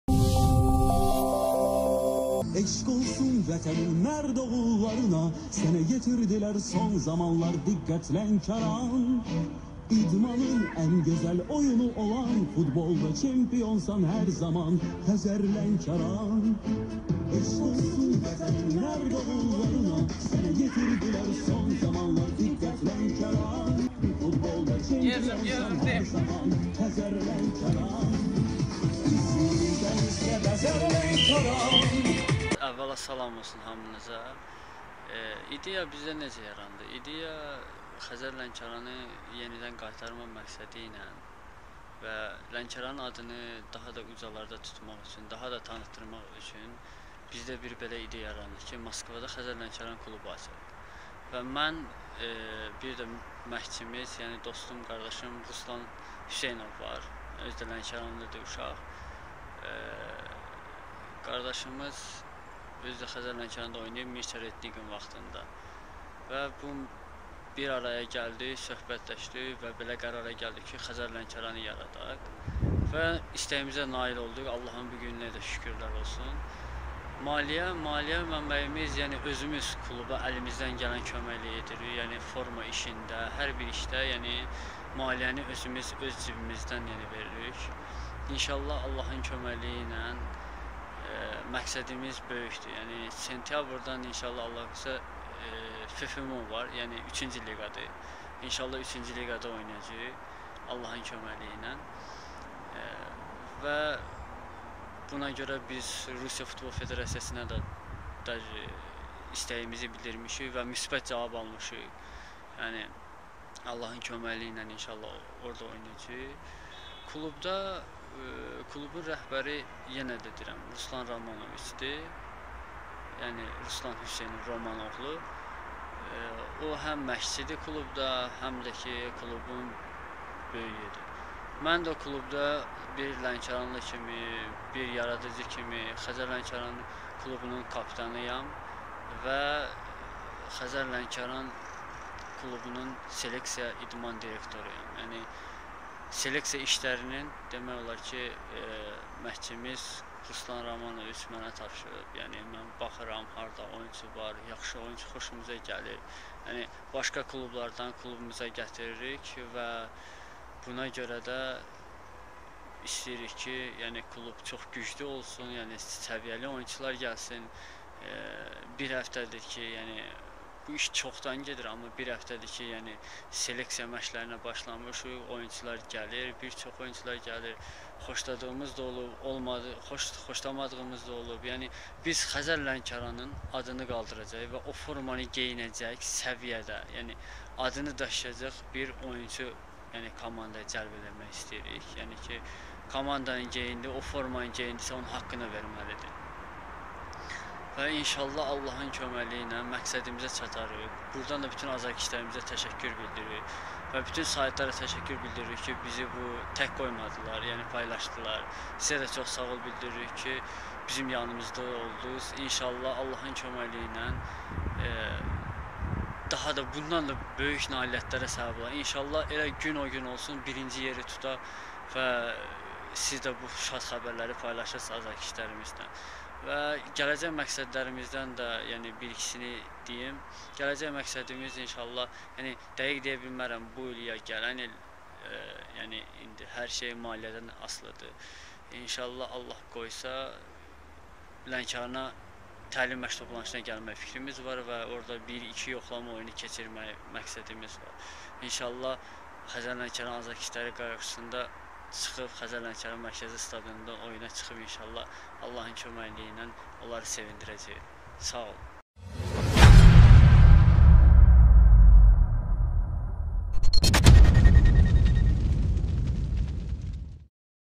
"Xəzər-Lənkəran formasını geyinən oyunçu o formanın haqqını verməlidir"-Klub kapitanı Elnur Babayev
"Xəzər Lənkəran Moskva"-nın seleksioneri Elnur Babayev:"Biz istədik Moskvada da Lənkəranı tanıdaq, ona görə də bunu yaratdıq. Maliyyəni öz cibimizdən veririk. Məqsədimiz böyükdür. 3-cü liqada oynayacağıq. Komandaya yeni oyunçular yığırıq, "Xəzər Lənkəran"-ın adına layiq oyunçular olmalıdır, o formanın haqqını verməlidir".